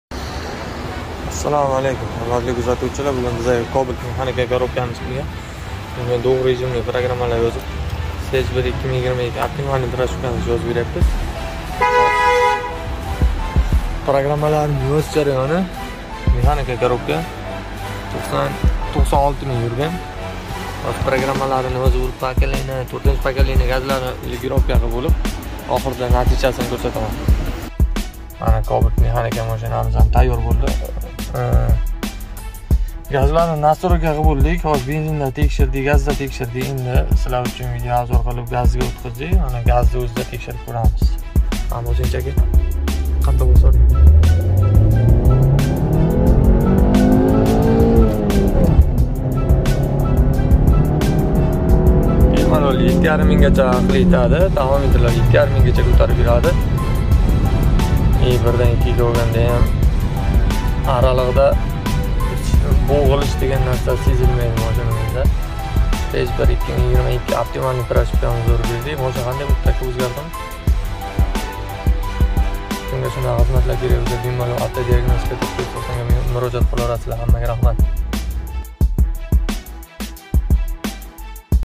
Cobalt MT Stage 1 2022 New
🚘Cobalt MT yangi Stage 1 2022 programma qilib berildi

🚘Stage proshivkalar📈⚡️🚀
💻AKPP ECU BSM proshivkalar📈
💻GBO-4 proshivkalarni to'g'irlash 📈
🔧GBO-3-4 tamirlash
🔧Injiktor tozalash
🔧 GBO-3-4 ustanovka